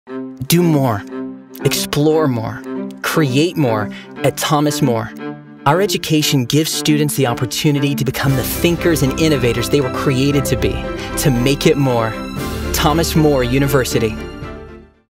Thomas More University. Make It More.
Thomas More University provides students with something much more than job training. Our graduates are prepared for any career. But they leave here prepared for much more.